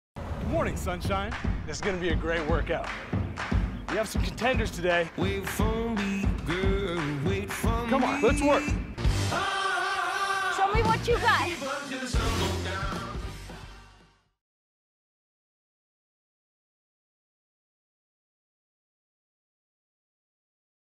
Live Workout FROM HOME l Cardio & Strength l Quantum
3..2...1... GO      Come join the rest of the iFit community and workouts LIVE.  If your quarantine days are starting to blur, try a new workout in a different location every day.